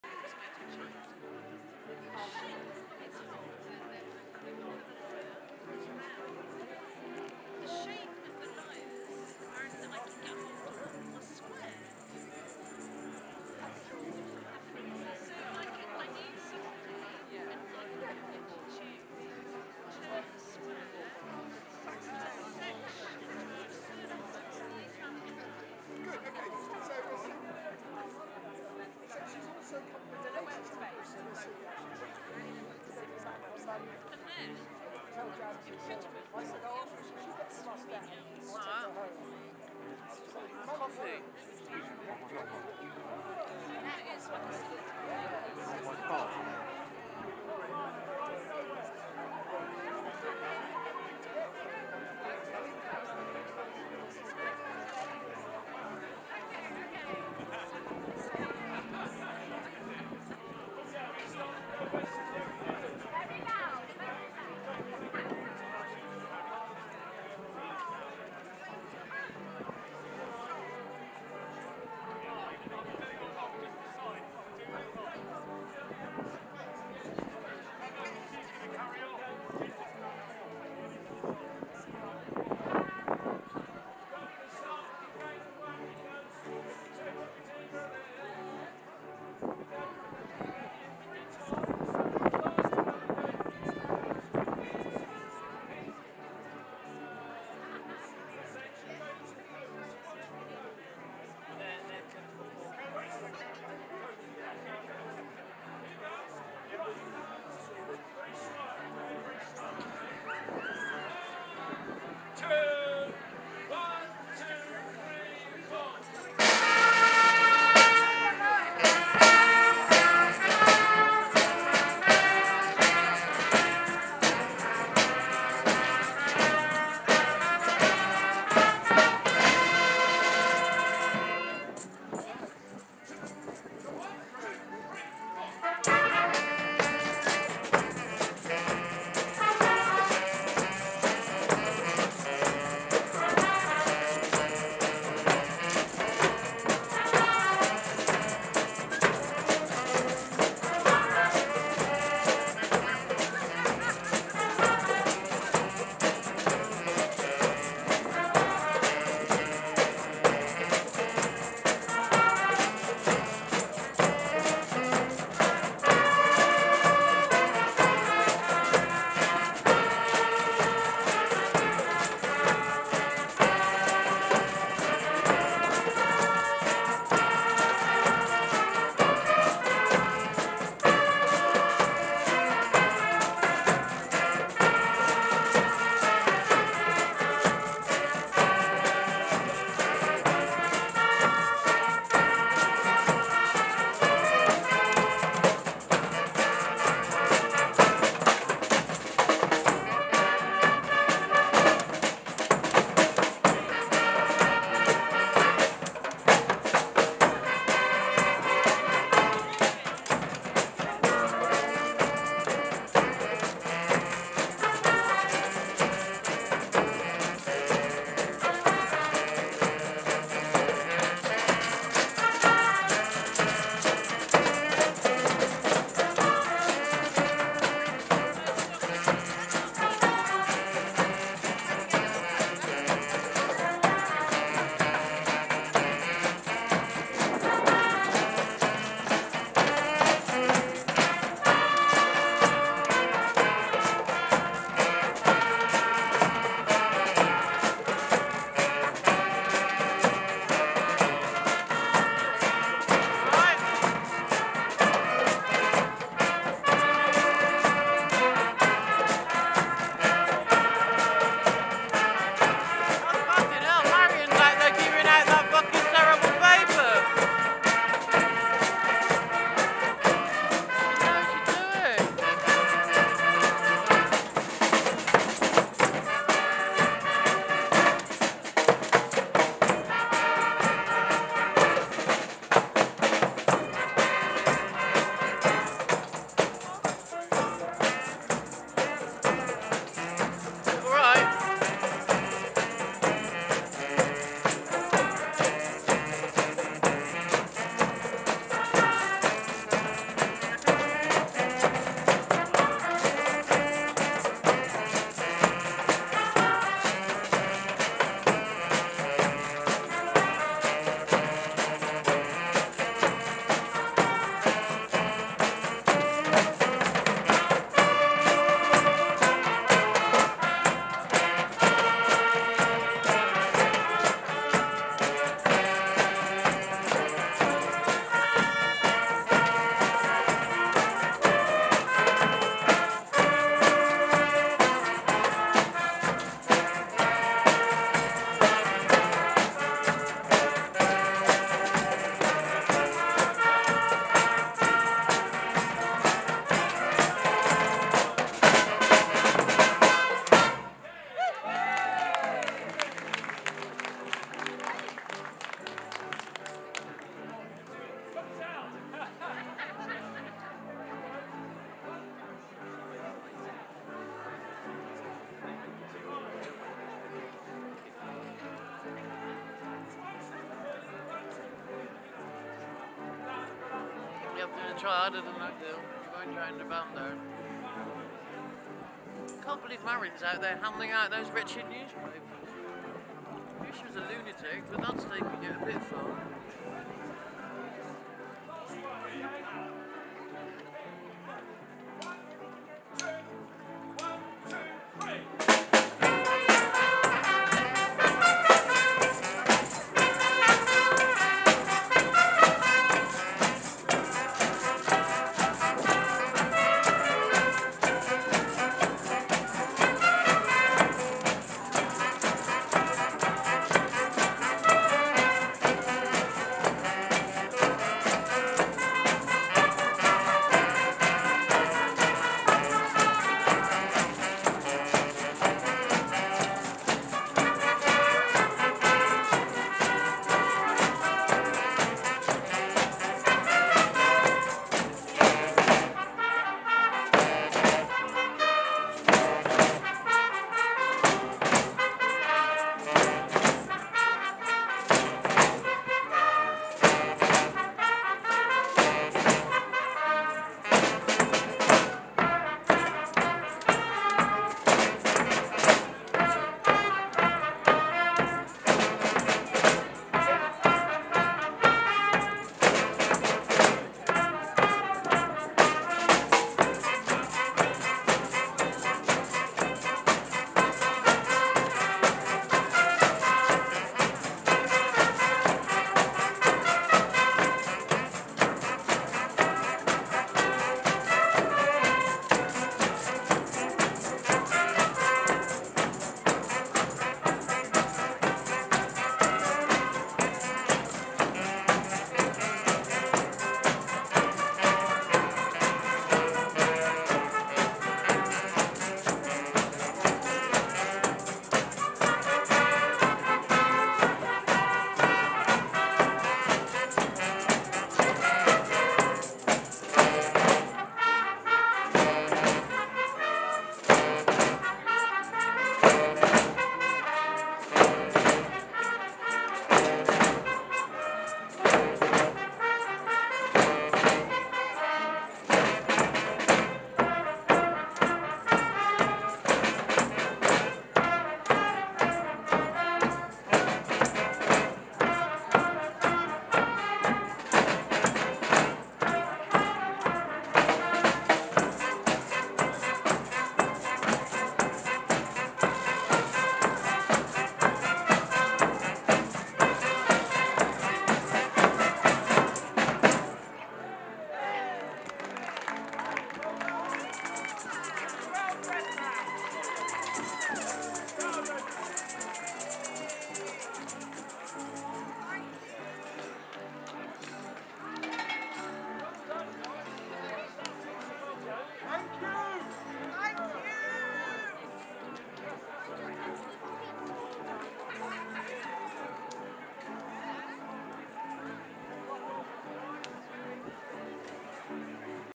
Stroud Red Band
The Red Band busking on Stroud High Street, on Saturday the 29th January 2022, to collect money for Refugee Aid Stroud, , and to raise awareness of Holocaust Memorial day.